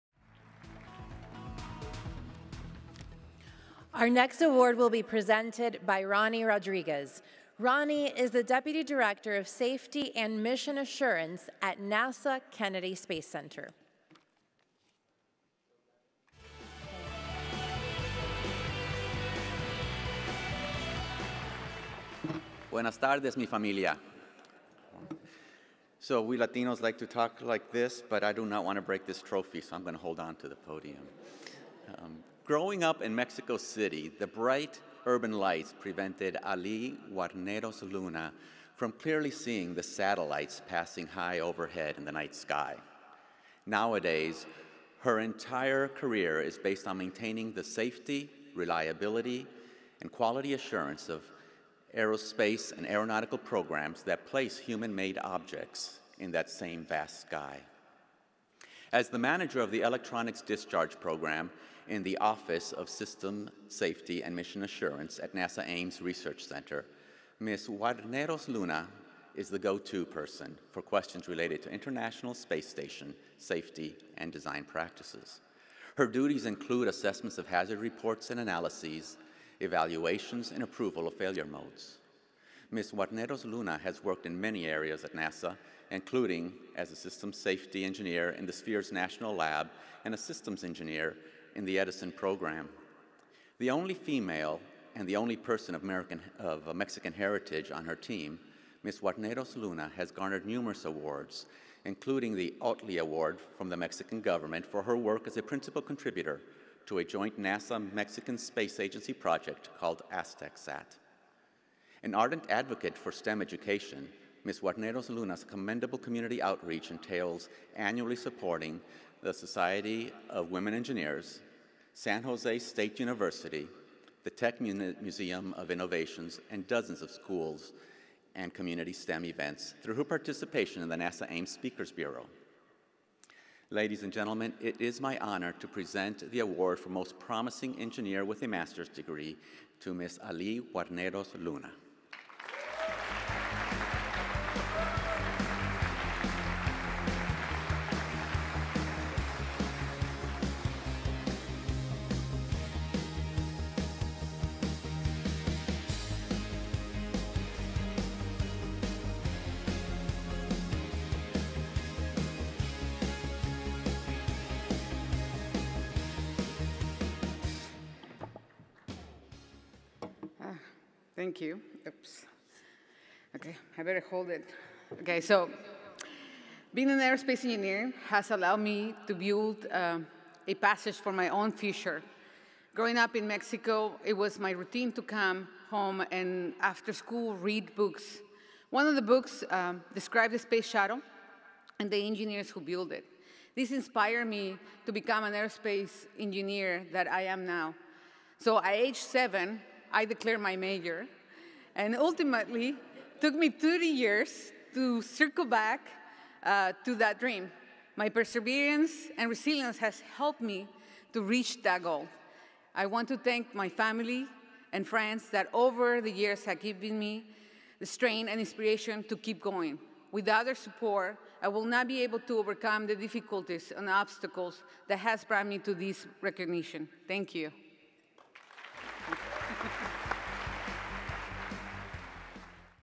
2019 Bridge to Tomorrow Luncheon Part 6
Ronnie Rodriguez, Deputy Director Safety and Mission Assurance from NASA Kennedy Space Center with 2019 Most Promising Engineer Master's Award Recipient Ali Guarneros-Luna, Manager Electronics Discharge Program, NASA Ames Research Center.